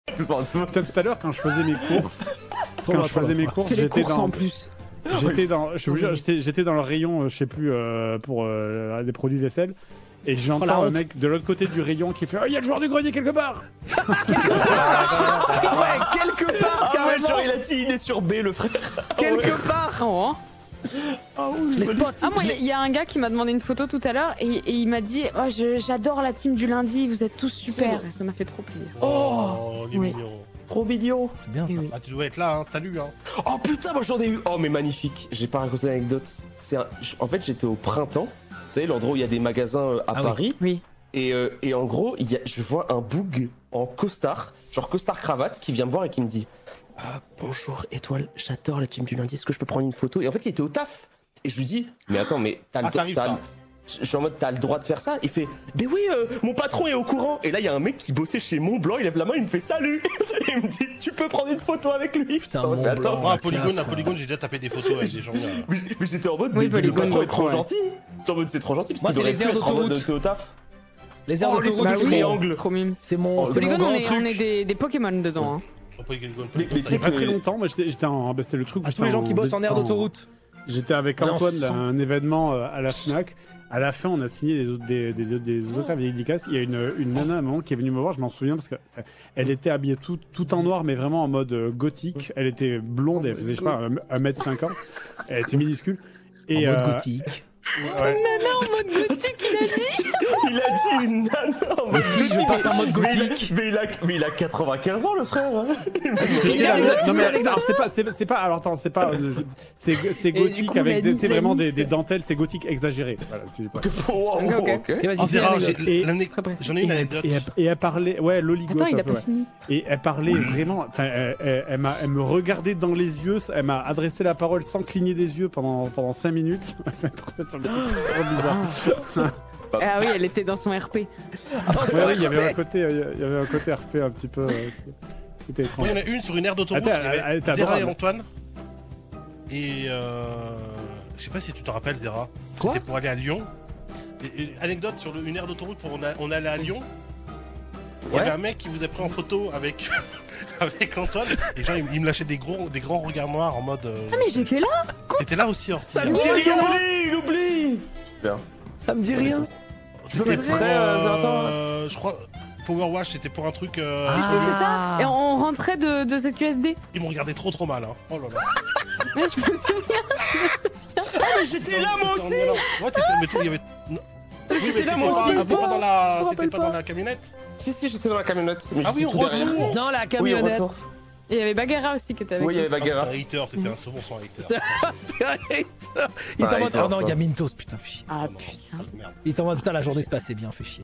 Anecdotes IRL de JDG et la team sur des viewers [Just Chatting]
Soirée du lundi sur Twitch.

Si vous aimez minecraft vous pouvez rejoindre le serveur de la chaîne LundiCraft dont voici une rapide vidéo de présentation

(N'hésitez pas à me dire si j'en oublie)